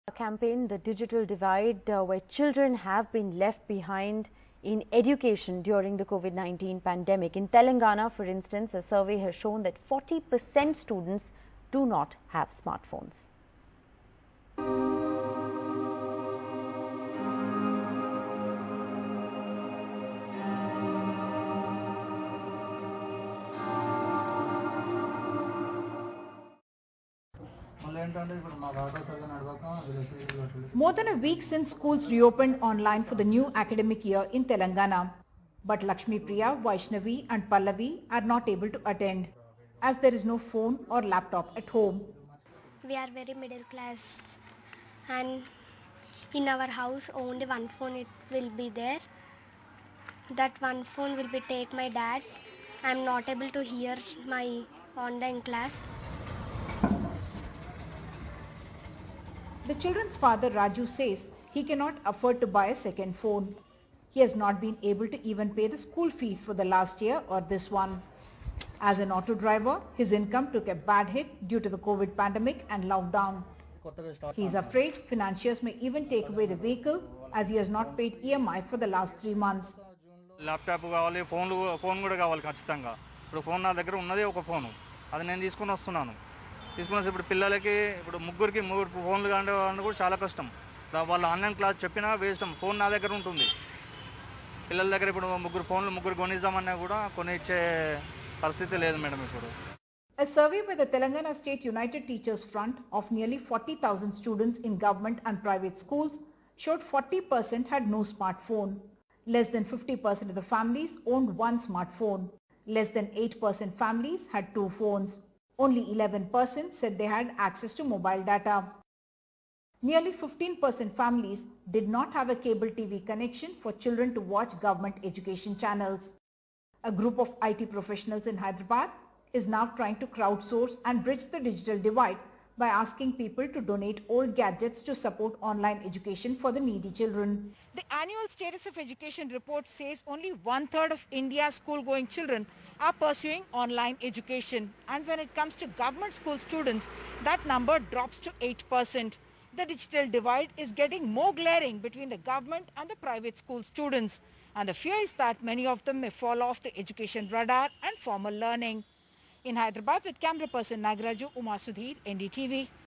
40% Students In Telangana Don't Have Smartphones, Shows Survey
A huge digital divide has been revealed in India during the COVID-19 pandemic, especially among students. In Telangana, a survey showed that 40 per cent of students don't have smartphones. A report on the plight of students in Telangana and how their education has been affected.